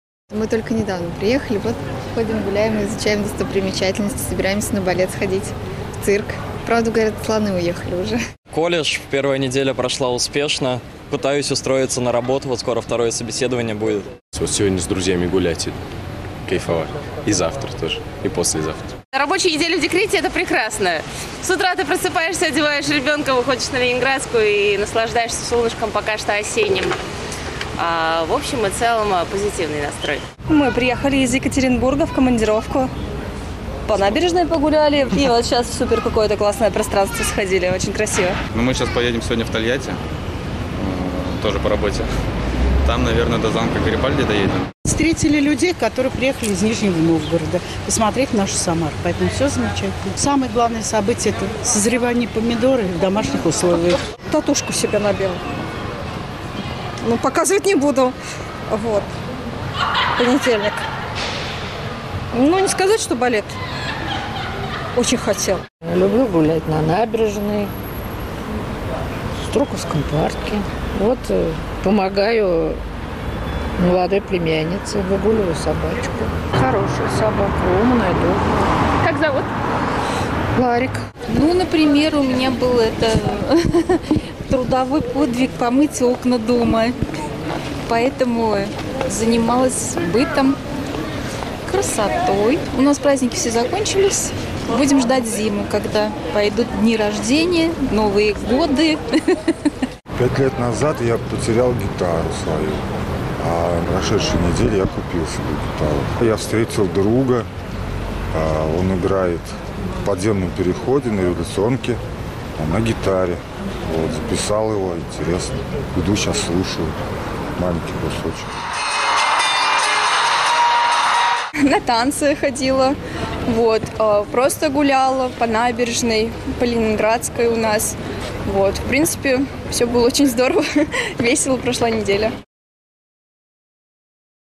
Чем запомнилась самарцам и гостям города уходящая рабочая неделя
Набили тату, собрали урожай помидоров и купили новую гитару. Чем ещё запомнилась самарцам и гостям города уходящая рабочая неделя, какие планы они уже построили на выходные, - выяснила наш корреспондент Ксения Баканова.